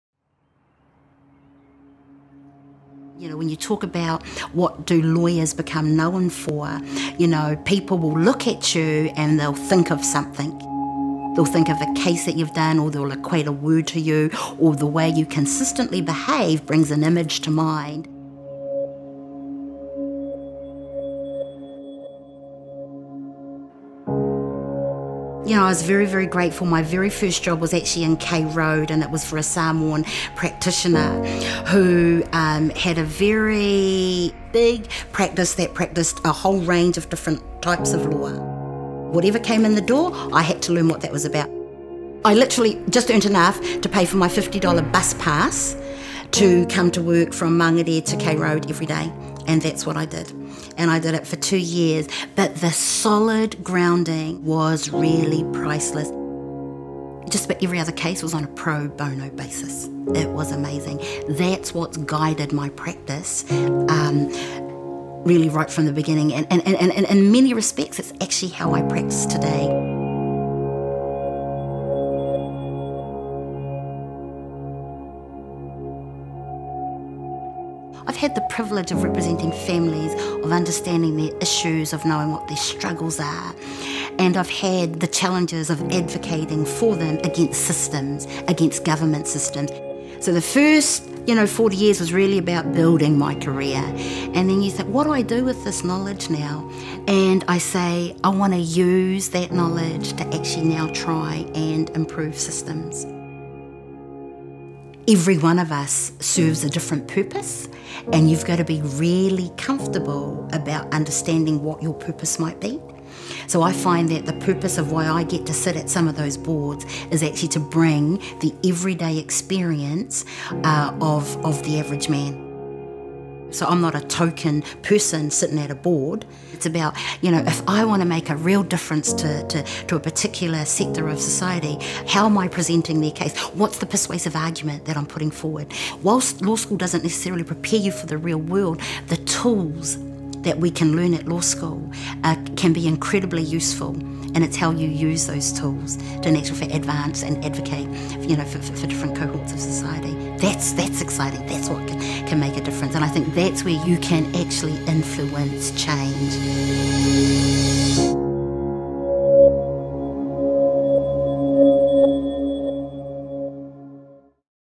"Every one of us serves a different purpose"
With decades of experience advocating for families, Ali'imuamua Sandra Alofivae now uses her skills and knowledge to improve policy at a government and regional level.